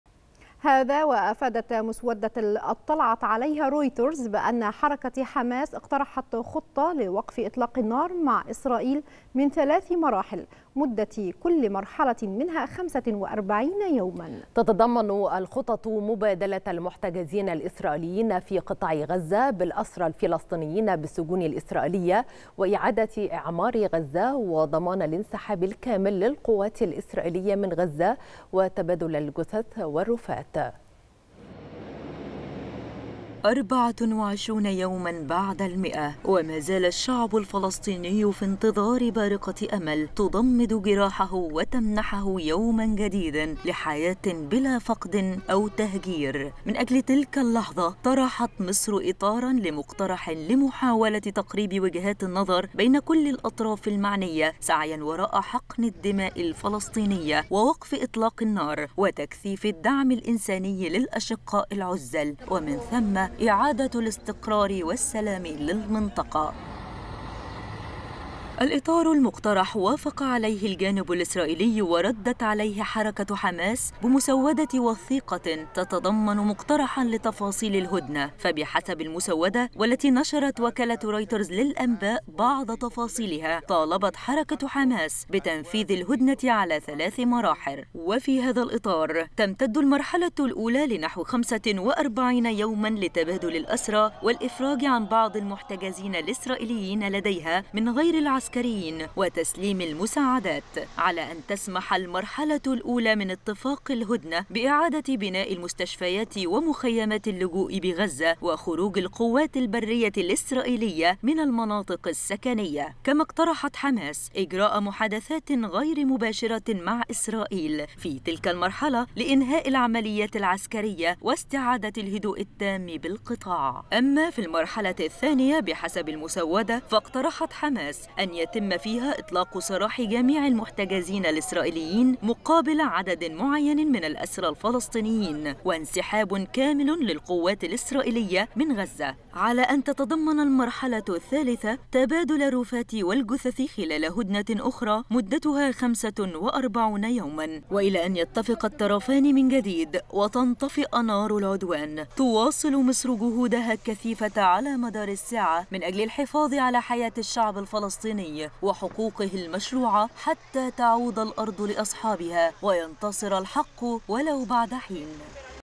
نشرة الثالثة | مصر تواصل جهود الوساطة لإنجاز وقف لإطلاق النار في غزة
نشرة الثالثة | مصر تواصل جهود الوساطة لإنجاز وقف لإطلاق النار في غزة#نشرة_الثالثة